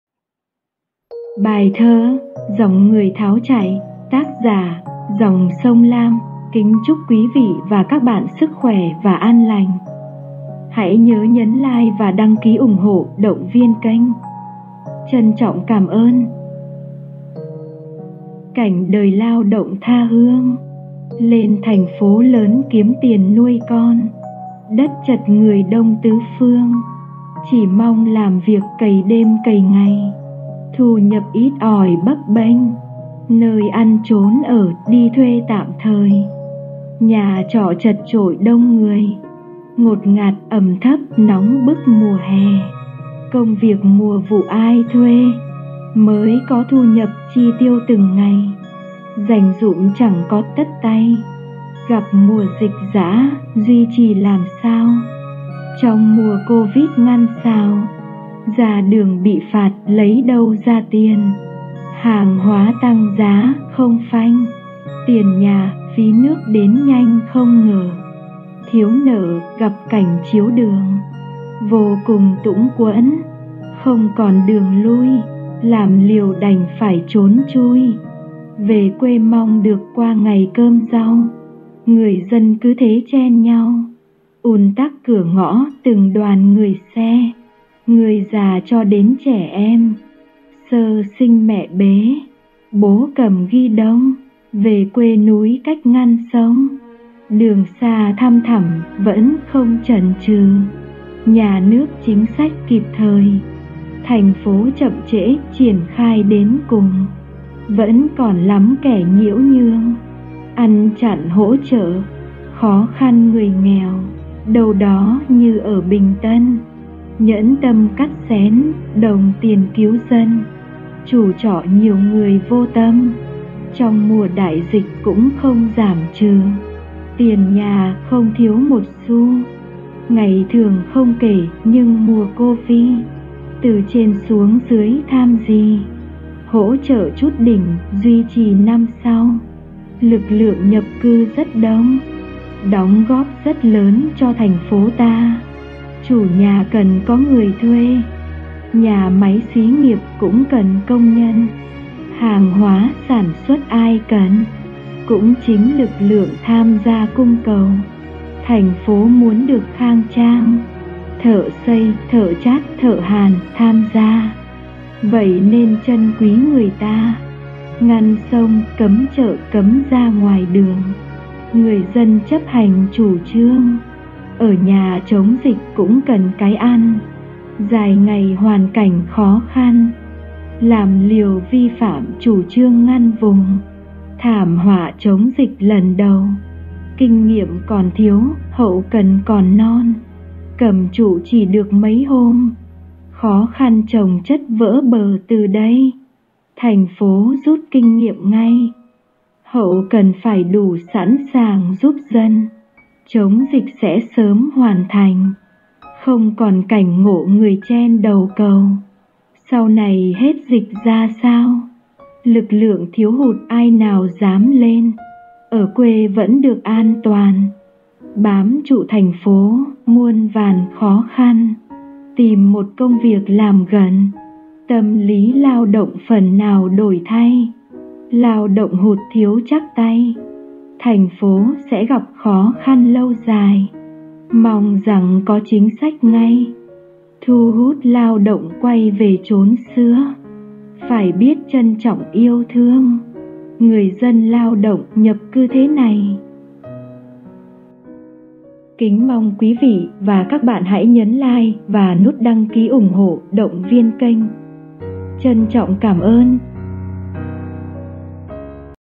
CẢM THƯƠNG, DÒNG NGƯỜI THÁO CHAY KHỎI THÀNH PHỐ VÌ SỢ ĐÓI!
KÍNH CHÚC QUÝ VỊ VÀ CÁC BẠN SỨC KHỎE, HẠNH PHÚC VÀ GIA ĐÌNH AN LÀNH!
Cảnh đời lao động tha hương.
Lên thành phố lớn kiếm tiền nuôi con.
Đất chật người đông tứ phương.
Chỉ mong làm việc, cày đêm cày ngày.
Thu nhập ít ỏi bấp bênh.
Nơi ăn chốn ở, đi thuê tạm thời.
Nhà trọ chật chội đông người.
Ngột ngạt, ẩm thấp, nóng bức mùa hè.
Công việc mùa vụ ai thuê.
Mới có thu nhập, chi tiêu từng ngày.
Dành dụm chẳng có, tất tay.
Gặp mùa dịch dã, duy trì làm sao!
Trong mùa cô vít ngăn sào.
Ra đường bị phạt, lấy đâu ra tiền?
Hàng hóa tăng giá không phanh!
Tiền nhà, phí nước đến nhanh không ngờ.
Thiếu nợ, gặp cảnh chiếu đường.
Vô cùng tũng quẫn, không còn đường lui.
Làm liều đành phải trốn chui.
Về quê mong được, qua ngày cơm rau.
Người dân cứ thế chen nhau.
Ùn tắc cửa ngõ, từng đoàn người xe.
Người già cho đến trẻ em.
Sơ sinh mẹ bế, bố cầm ghi đông.
Về quê núi cách ngăn sông.
Đường xa thăm thẳm, vẫn không chần chừ.
Nhà nước chính sách kịp thời.
Thành phố chậm trễ, triển khai đến cùng.
Vẫn còn lắm kẻ nhiễu nhương.
Ăn chặn hỗ trợ, khó khăn người nghèo.
Đâu đó như ở Bình Tân!
Nhẫn tâm cắt xén, đồng tiền cứu dân!
Chủ trọ nhiều người vô tâm.
Trong mùa đại dịch, cũng không giảm trừ!
Tiền nhà không thiếu một xu.
Ngày thường không kể, nhưng mùa cô vi.
Từ trên xuống dưới tham gì?
Hỗ trợ chút đỉnh, duy trì năm sau!
Lực lượng nhập cư rất đông.
Đóng góp rất lớn cho thành phố ta.
Chủ nhà cần có người thuê.
Nhà máy xí nghiệp, cũng cần công nhân.
Hàng hóa sản xuất ai cần.
Cũng chính lực lượng tham gia cung cầu.
Thành phố muốn được khang trang.
Thợ xây, thợ trát, thợ hàn, tham gia.
Vậy nên trân quý người ta.
Ngăn sông, cấm chợ, cấm ra ngoài đường.
Người dân chấp hành chủ trương.
Ở nhà chống dịch, cũng cần cái ăn.
Dài ngày hoàn cảnh khó khăn.
Làm liều vi phạm chủ trương ngăn vùng.
Thảm họa chống dịch lần đầu.
Kinh nghiệm còn thiếu, hậu cần còn non.
Cầm trụ chỉ được mấy hôm.
Khó khăn chồng chất vỡ bờ từ đây.
Thành phố rút kinh nghiệm ngay.
Hậu cần phải đủ sẵn sàng giúp dân.
Chống dịch sẽ sớm hoàn thành.
Không còn cảnh ngộ người chen đầu cầu.
Sau này hết dịch ra sao?
Lực lượng thiếu hụt, ai nào giám lên!
Ở quê vẫn được an toàn.
Bám trụ thành phố, muôn vàn khó khăn!
Tìm một công việc làm gần.
Tâm lý lao động phần nào đổi thay.
Lao động hụt thiếu chắc tay.
Thành phố sẽ gặp khó khăn lâu dài.
Mong rằng có chính sách ngay.
Thu hút lao động quay về chốn xưa.
Phải biết trân trọng yêu thương.
Người dân lao động, nhập cư thế này.